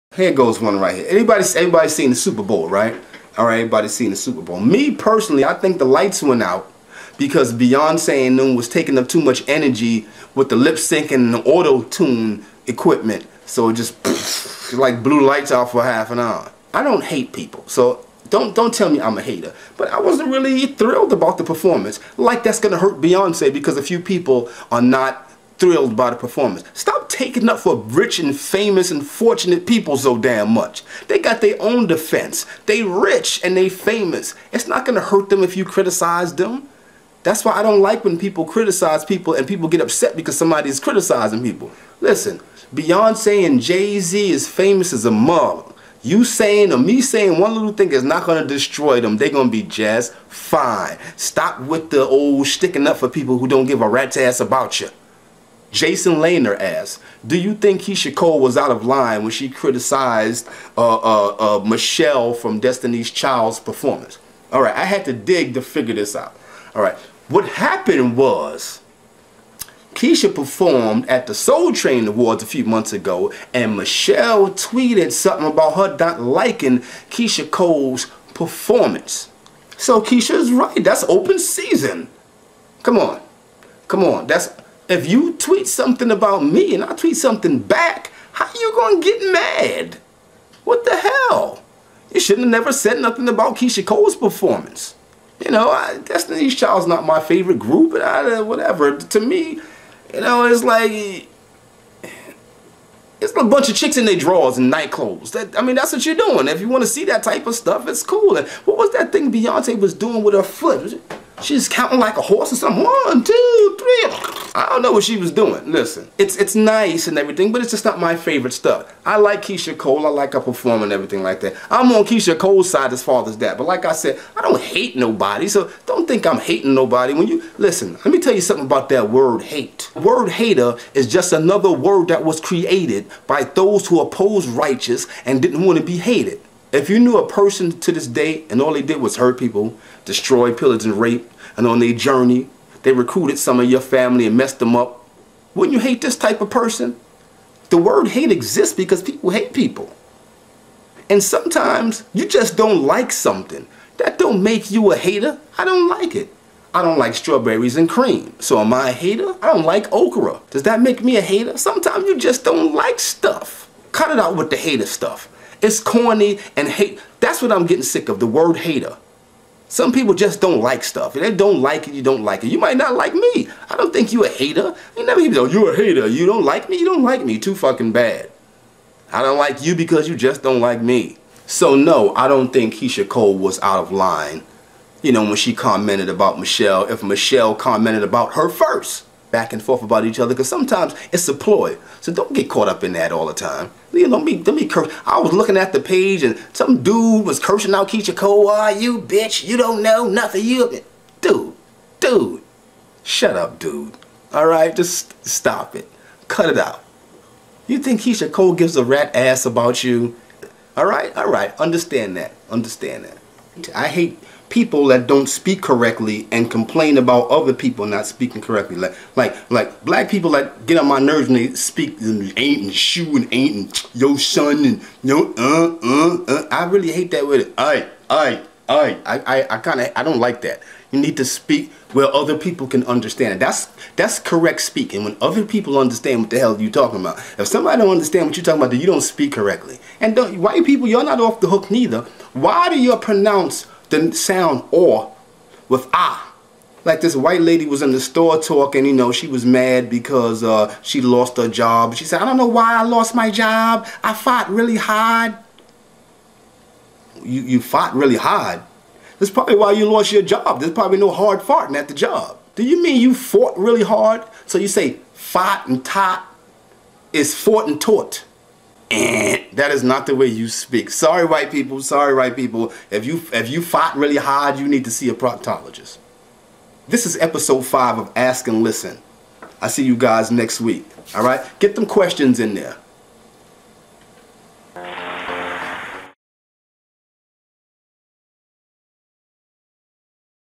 Ask And Listen. With Dre Parker. Ep 5 PART 2 of 2
Ask And Listen. With Dre Parker. Ep 5 PART 2 of 2 please feel fee to share and re-post.
I Talk about the Superbowl, Beyonce' and KeyshIa Cole